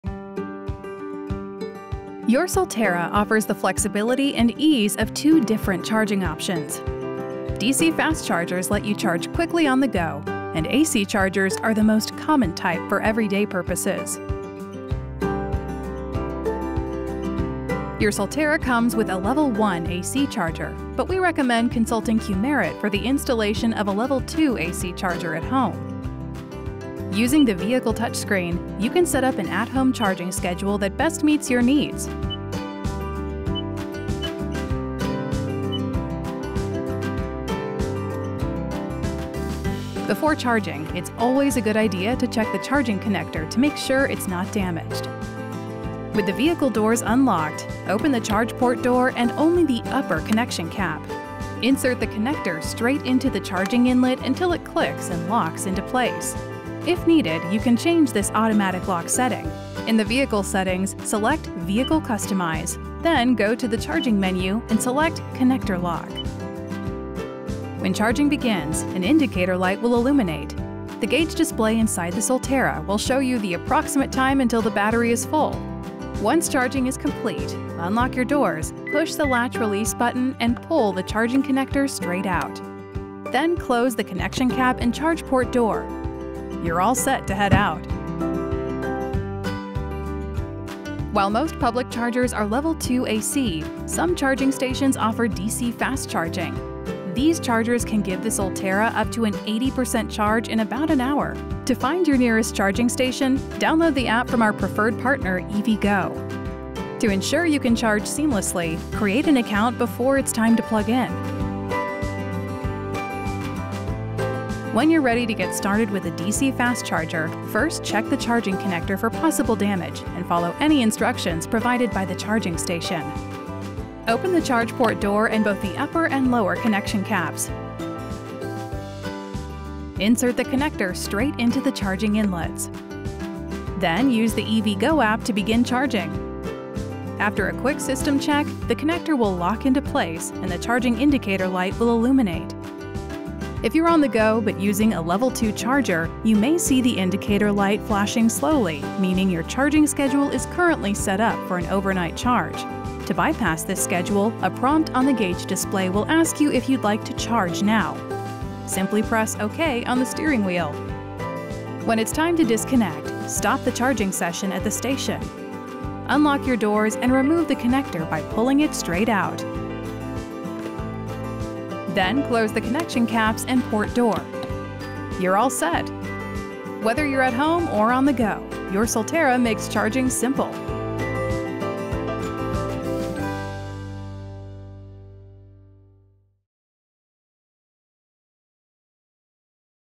How-To Charge Your EV | All-New 2023 Subaru Solterra
With the flexibility of AC and DC charging options, your all-electric Subaru Solterra makes it easy to plug in at home or on the go. Follow the simple steps in this video to get set up and begin charging seamlessly.